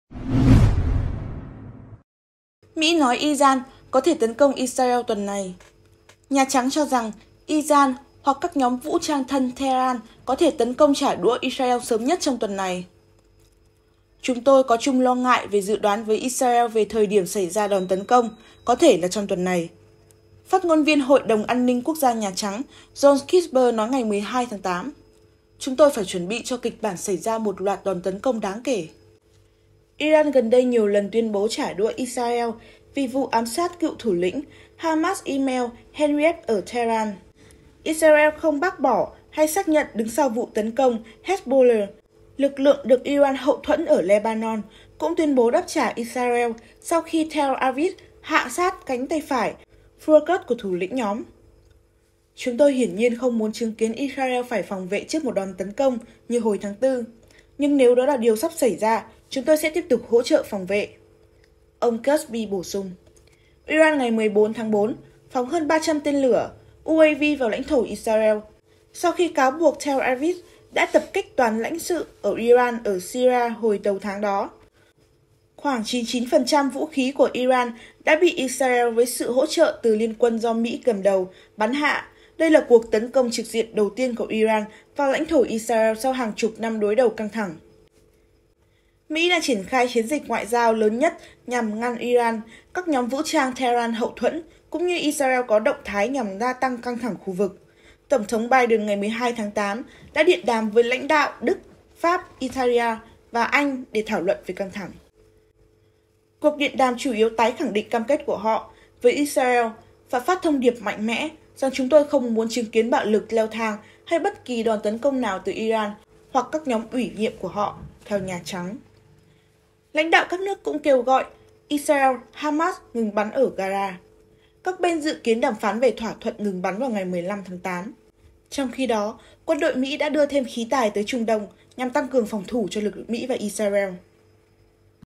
Mỹ nói Iran có thể x.ử đ.ẹ.p Israel tuần này | Sự Kiện & Bình Luận
Nhà Trắng cho rằng Iran hoặc các nhóm vũ trang thân Tehran có thể tấn công trả đũa Israel sớm nhất trong tuần này.

"Chúng tôi có chung lo ngại và dự đoán với Israel về thời điểm xảy ra đòn tấn công - có thể là trong tuần này", phát ngôn viên Hội đồng An ninh Quốc gia Nhà Trắng John Kirby nói ngày 12/8. "Chúng tôi phải chuẩn bị cho kịch bản xảy ra một loạt đòn tấn công đáng kể".
🔹 Bản quyền thuộc về  @Sự Kiện & Bình Luận
🔹 Copyright by  @Sự Kiện & Bình Luận
🔹 Vui lòng không đăng lại video. Do not reup.